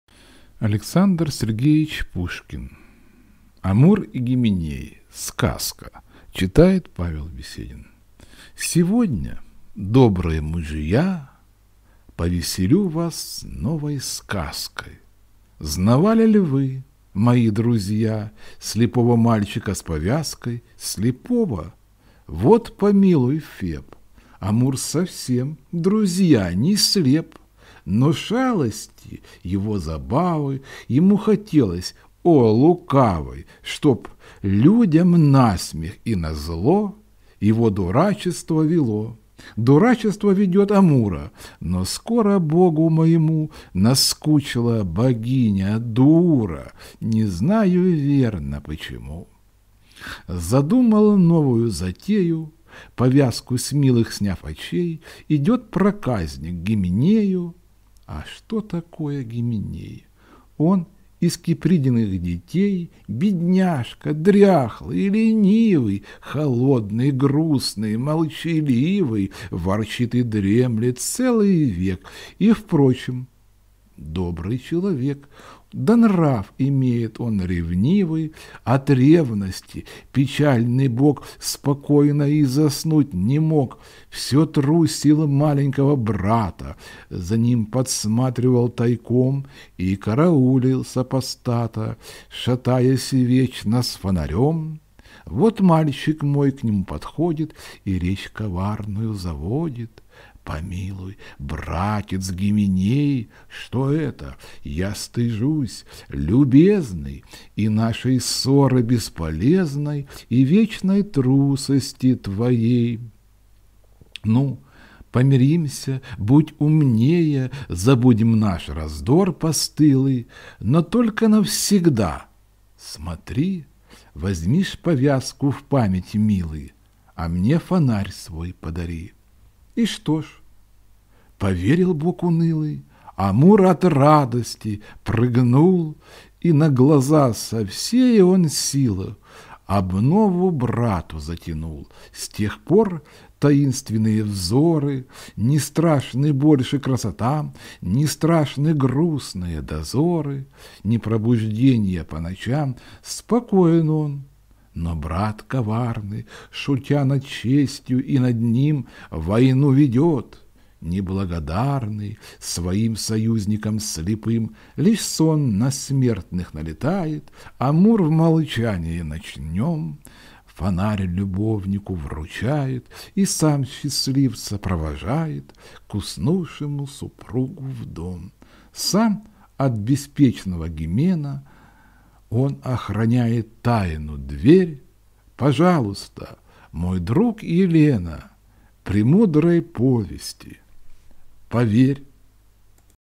Амур и Гименей , Александр Пушкин, Русская Поэзия, читает Павел Беседин
Сегодня, добрые мужья,
Повеселю вас новой сказкой.
Знавали ль вы, мои друзья,
Слепого мальчика с повязкой?
Слепого?.. Вот? Помилуй, Феб!
Амур совсем, друзья, не слеп:
Но шалуну пришла ж охота,
Чтоб, людям на смех и назло,
Его безумие вело.
Безумие ведет Эрота:
Но вдруг, не знаю почему,
Оно наскучило ему.
Взялся за новую затею:
Повязку с милых сняв очей,
Идет проказник к Гименею…
А что такое Гименей?
Он сын Вулкана молчаливый,
Холодный, дряхлый и ленивый,
Ворчит и дремлет целый век,
А впрочем добрый человек,
Да нрав имеет он ревнивый.
От ревности печальный бог
Спокойно подремать не мог;
Все трусил маленького брата,
За ним подсматривал тайком
И караулил супостата
С своим докучным фонарем.
Вот мальчик мой к нему подходит
И речь коварную заводит:
«Развеселися, Гименей!
Ну, помиримся, будь умней!
Забудь, товарищ мой любезный,
Раздор смешной и бесполезный!
Да только навсегда, смотри!
Возьми ж повязку в память, милый,
А мне фонарь свой подари!»
И что ж? Поверил бог унылый.
Амур от радости прыгнул,
И на глаза со всей он силы
Обнову брату затянул.
Гимена скучные дозоры
С тех пор пресеклись по ночам;
Его завистливые взоры
Теперь не страшны красотам;
Спокоен он, но брат коварный,
Шутя над честью и над ним,
Войну ведет неблагодарный
С своим союзником слепым.
Лишь сон на смертных налетает,
Амур в молчании ночном
Фонарь любовнику вручает
И сам счастливца провожает
К уснувшему супругу в дом;
Сам от беспечного Гимена
Он охраняет тайну дверь…
Пойми меня, мой друг Елена,
И мудрой повести поверь!